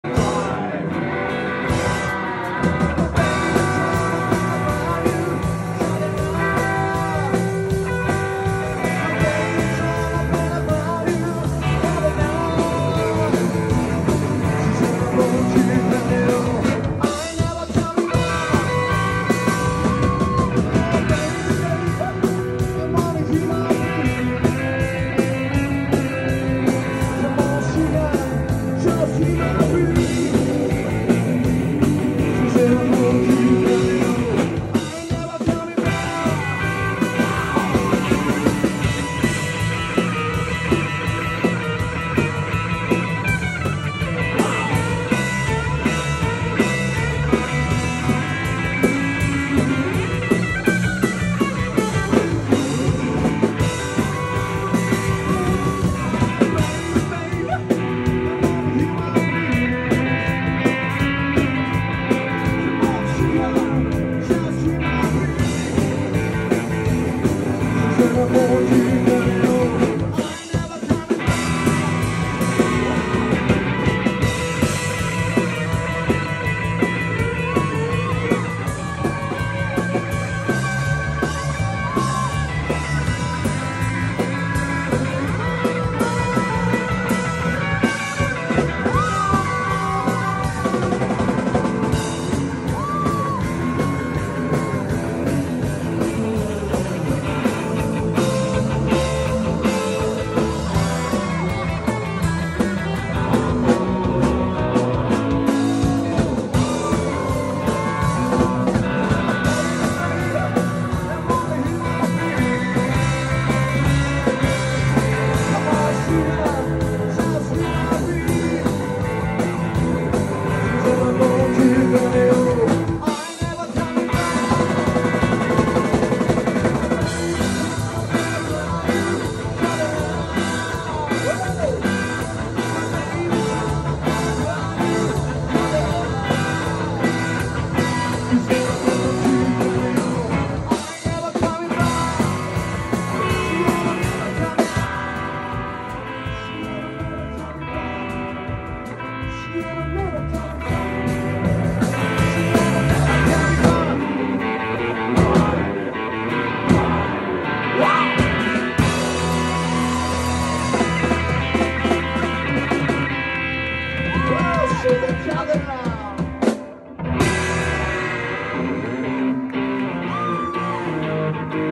THE ROCKRTZ 1
2023年9月24日に行われた　RockabillyWeekend!! BEAT GENERATION!!から
初登場私もお初の拝見です
マイクが入らなかったりトラブルだらけの中で頑張りました！